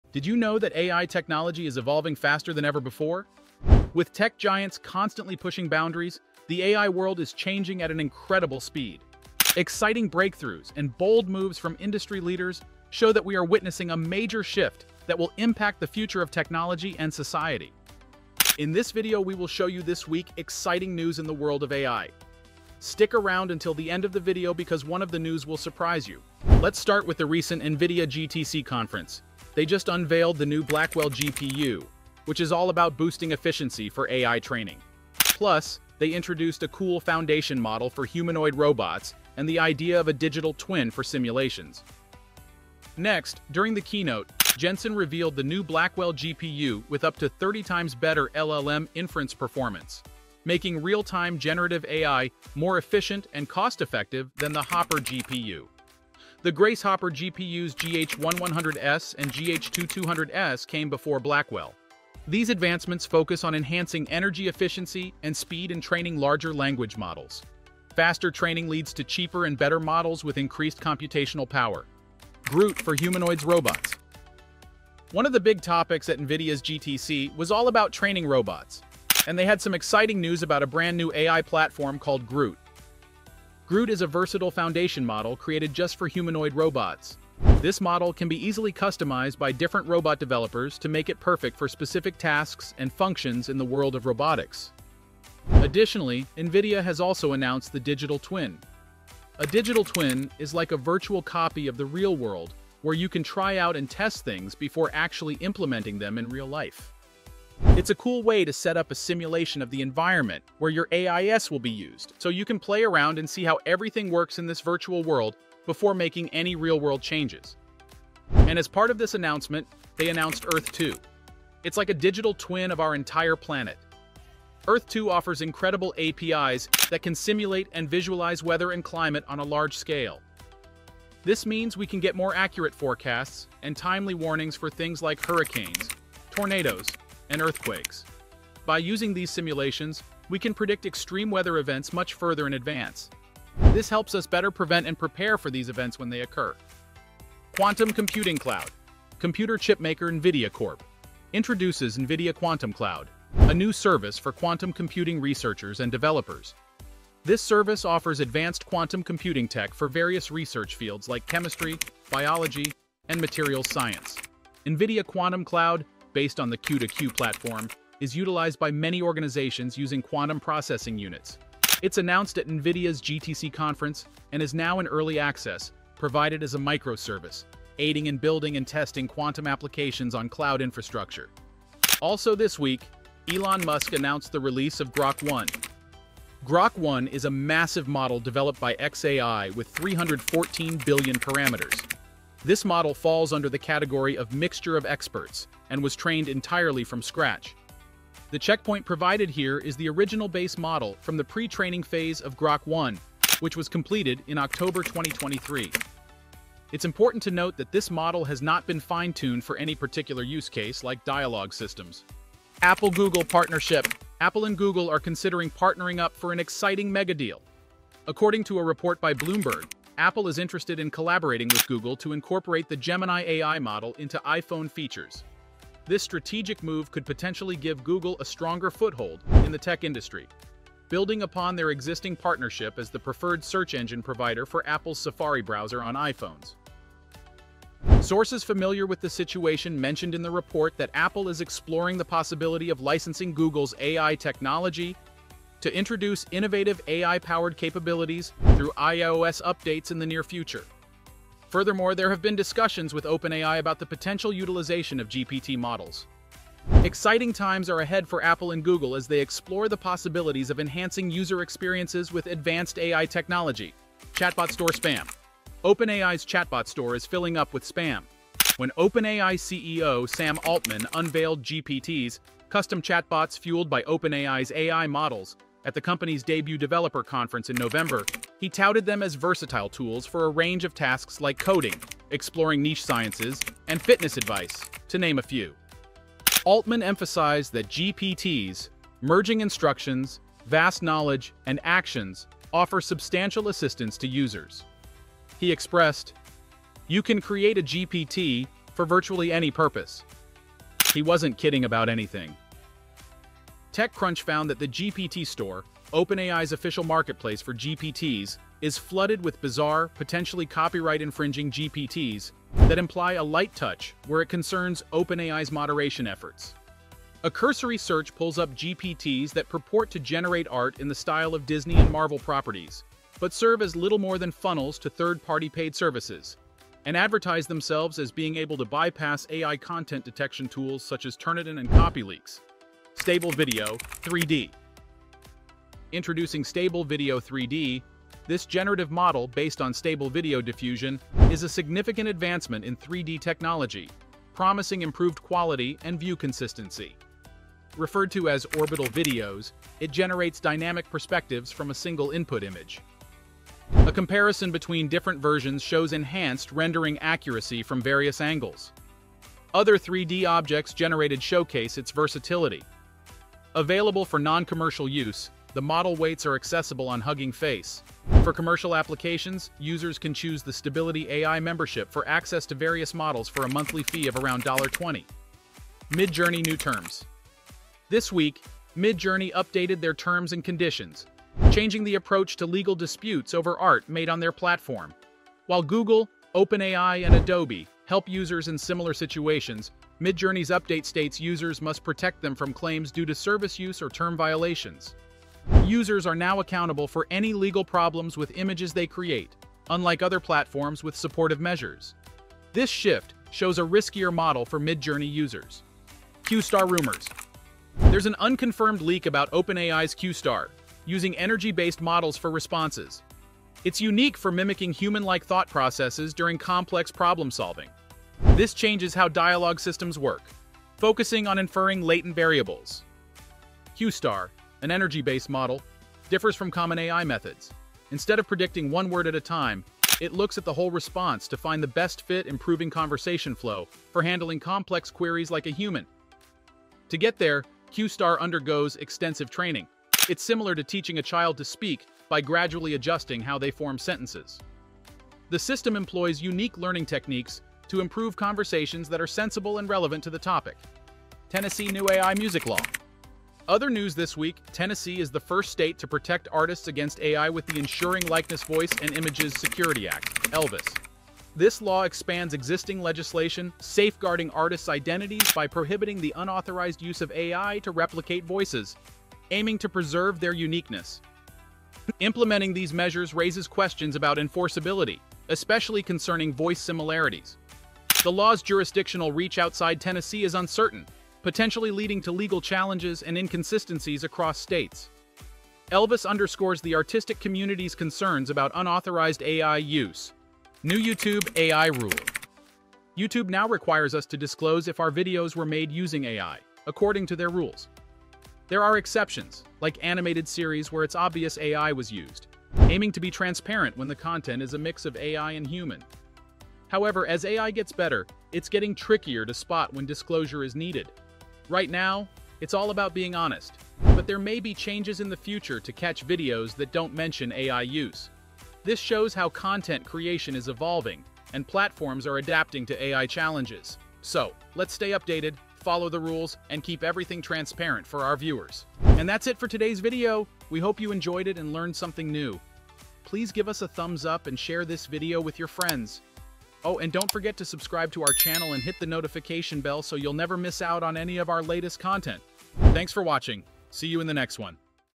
Revolutionizing AI: Latest Breakthroughs Unveiled
Did you know that AI technology is evolving faster than ever before?
With tech giants constantly pushing boundaries, the AI world is changing at an incredible speed. Exciting breakthroughs and bold moves from industry leaders show that we are witnessing a major shift that will impact the future of technology and society.

In this video, we will show you this week exciting news in the world of AI! Stick around until the end of the video because one of the news will surprise you!

In today's video we look at Revolutionizing AI: Latest Breakthroughs Unveiled

Inspired by Here's How Galaxy AI Is Changing The World Forever

Inspired by Here's How ChatGPT5 Will Change the World Forever

Inspired by The 5 AI Discoveries will Change the World Forever